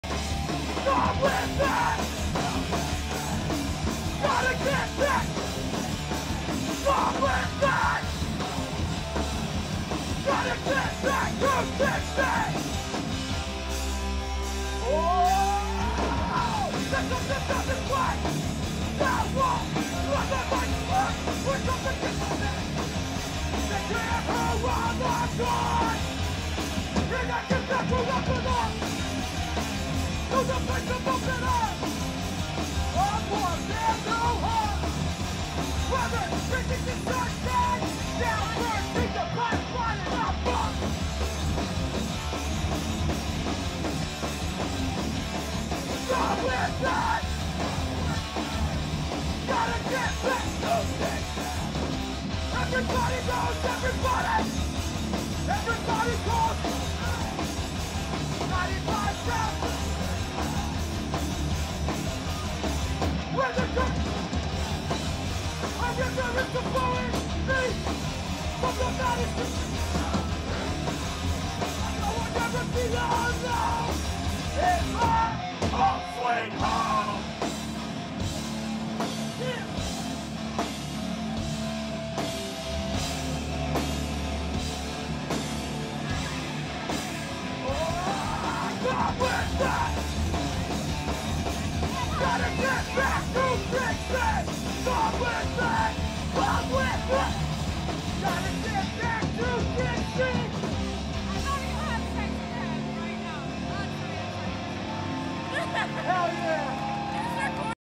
Down to Nothing - Live at Shakas in Va Beach
Had the privilege of recording these sets. Stoked on the way they came out. Sorry I didn't do much Post Processing. Will be trying to hook up with someone in the future to work together on that